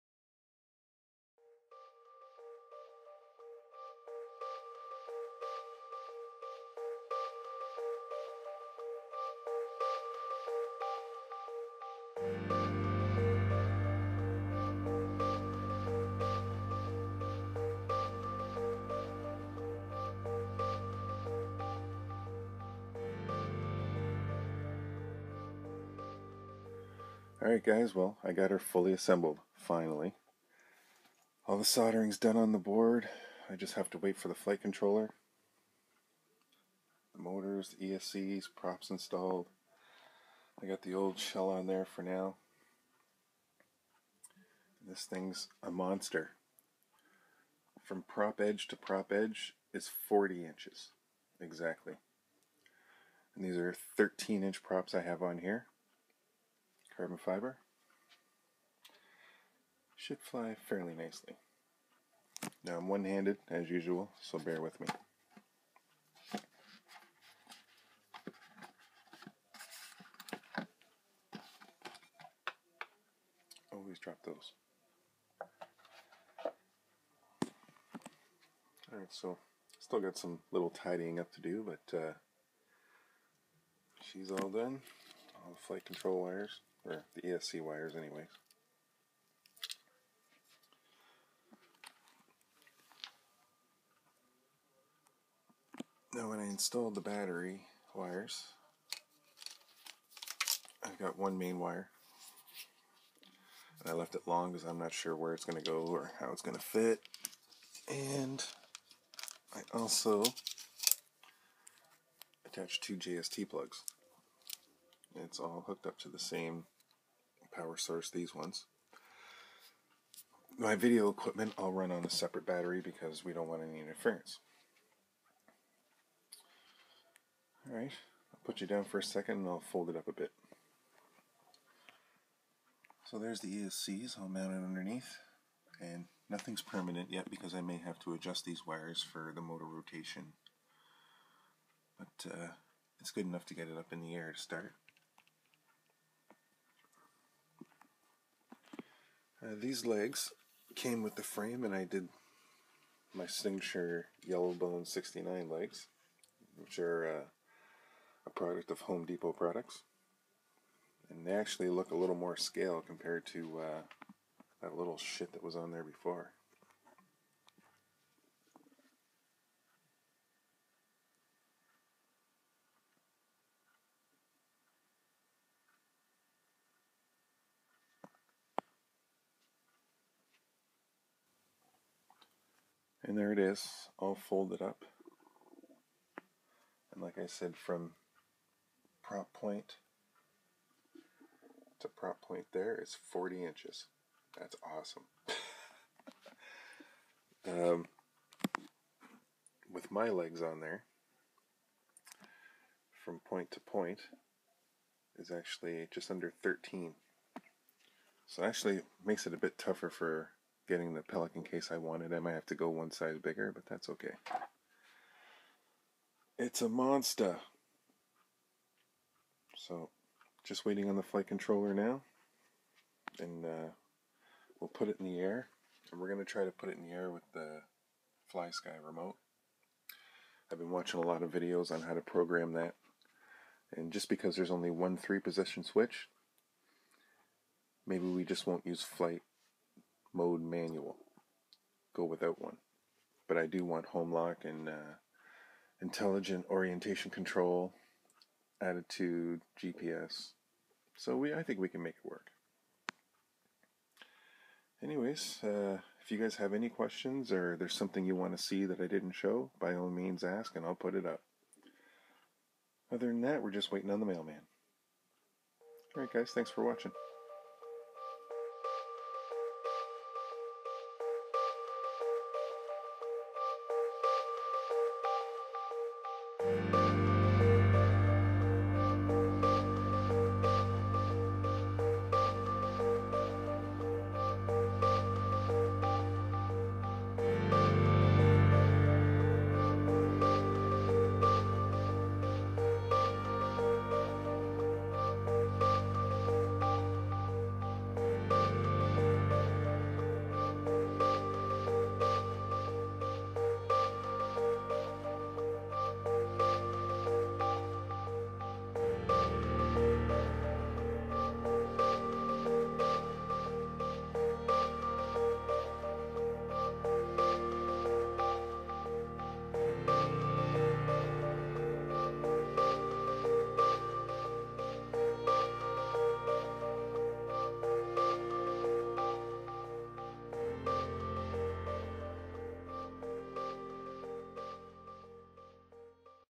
Tarot 680 Pro Hexacopter Build-Assembly Part 4
Well its all together and just waiting on the flight controller now.